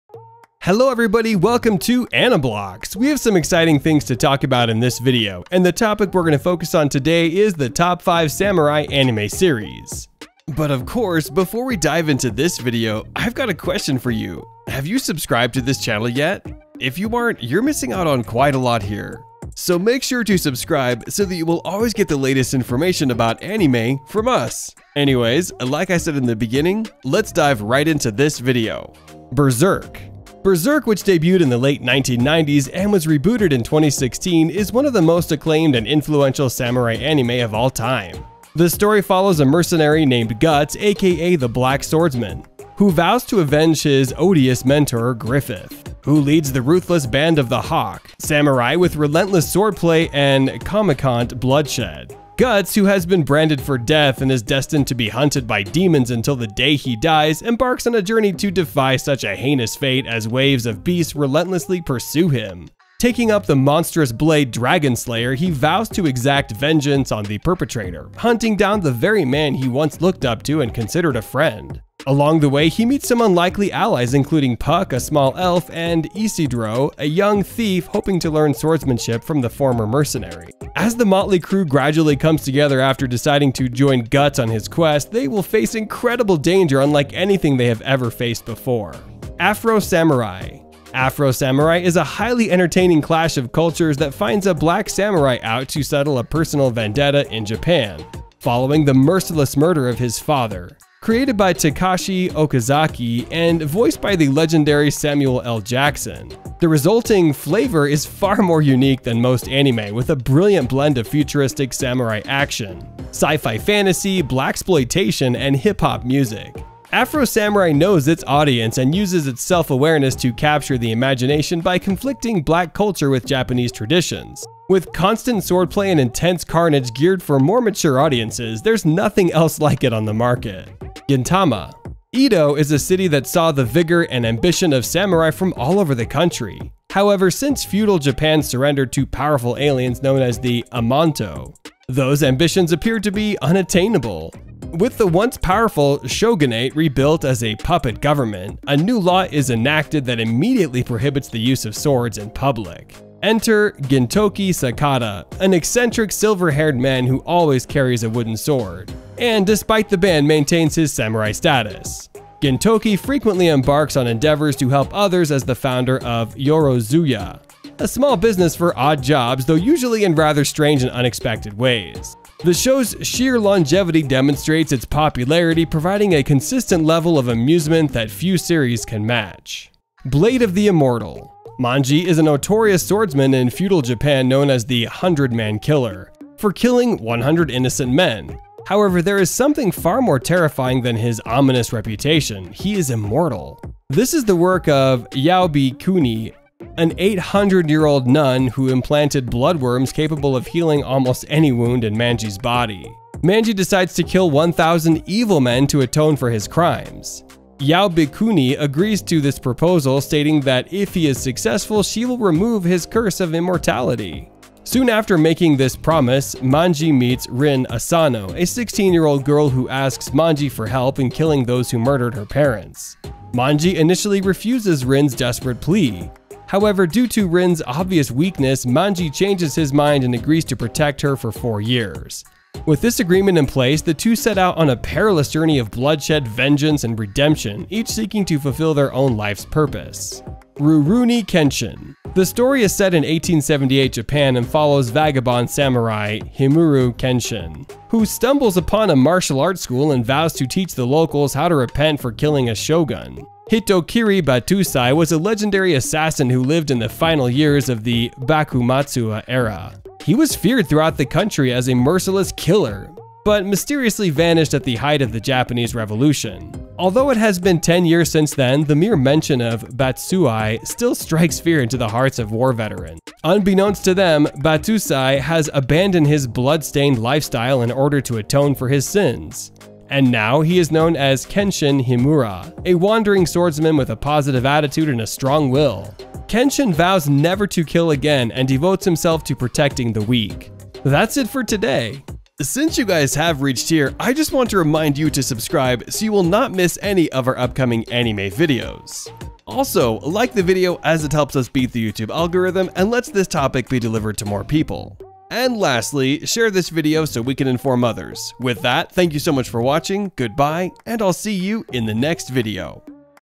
Top 5 Samurai Anime Series
In today's Blox Chain video we are going to be looking at the best samurai anime series!